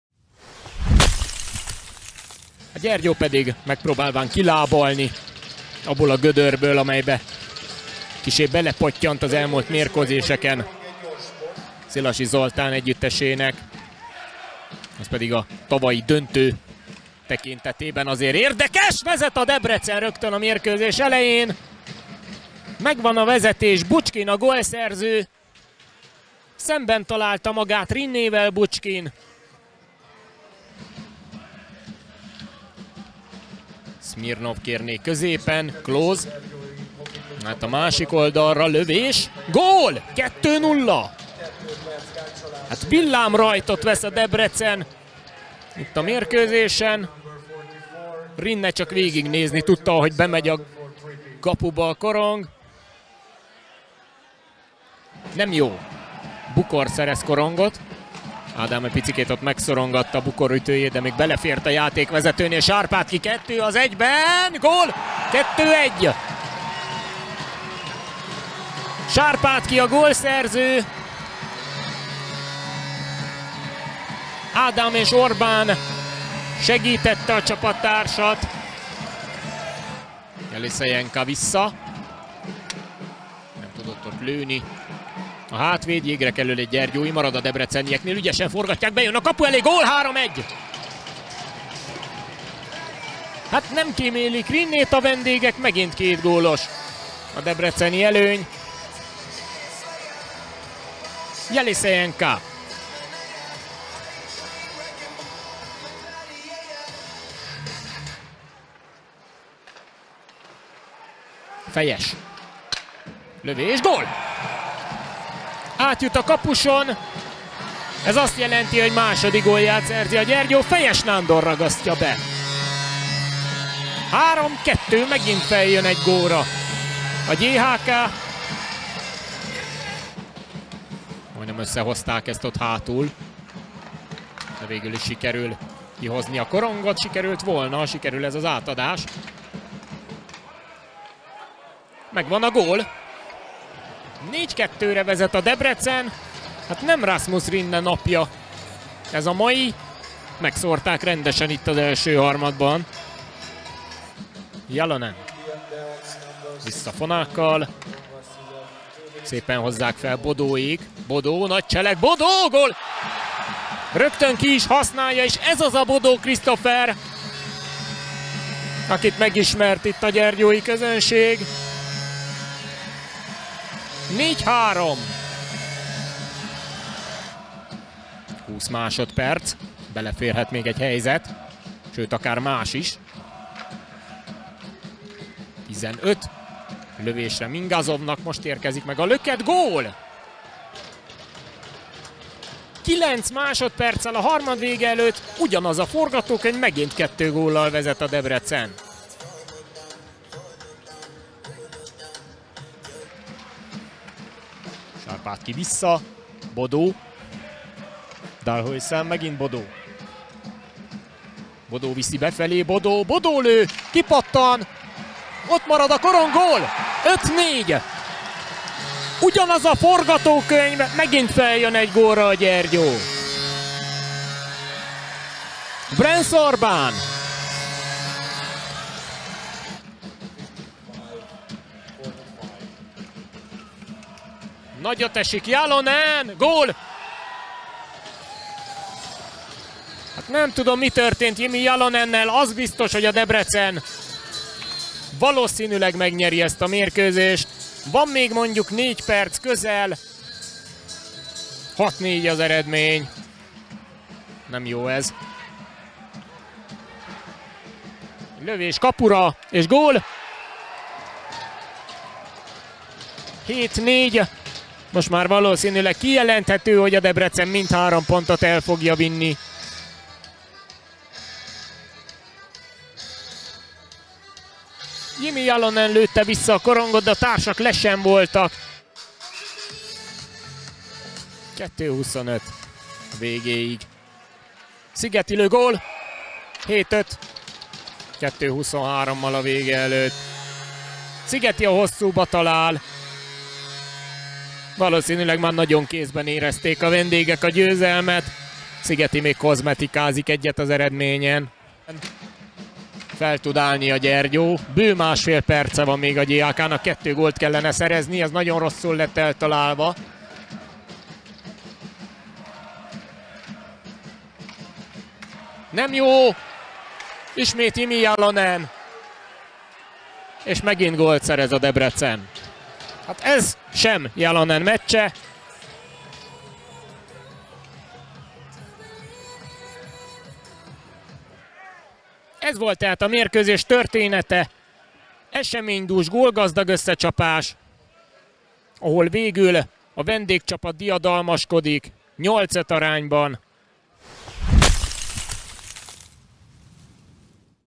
Gyergyói Hoki Klub - DEAC 5-8 (összefoglaló)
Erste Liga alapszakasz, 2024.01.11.